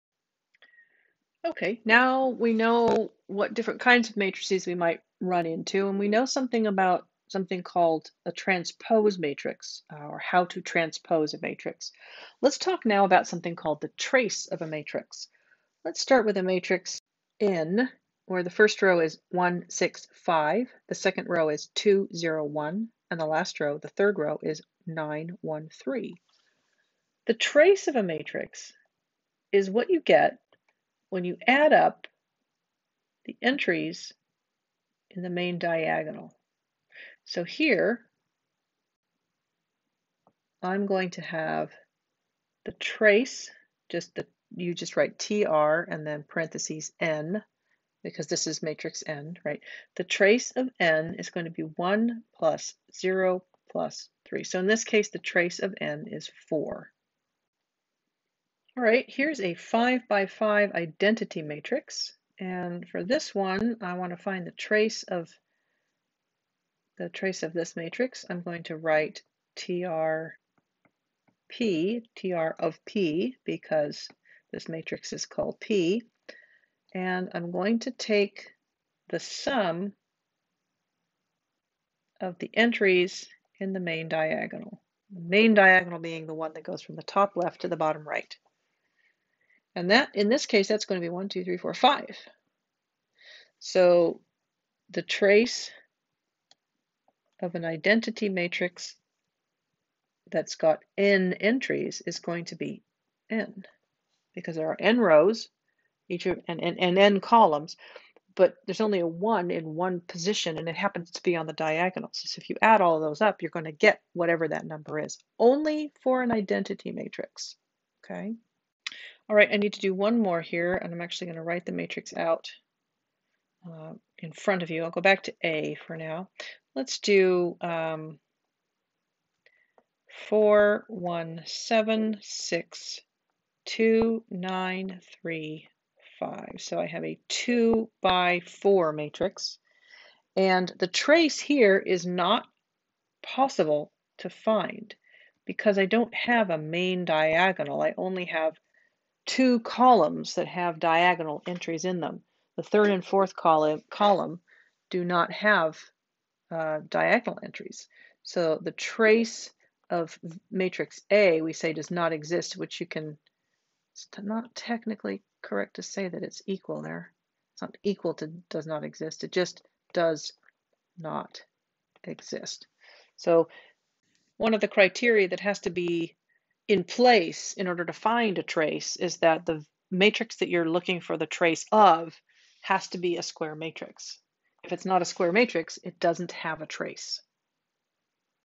Trace of a Matrix
The trace of a matrix is a quick addition problem, but it's important to know what we're adding up!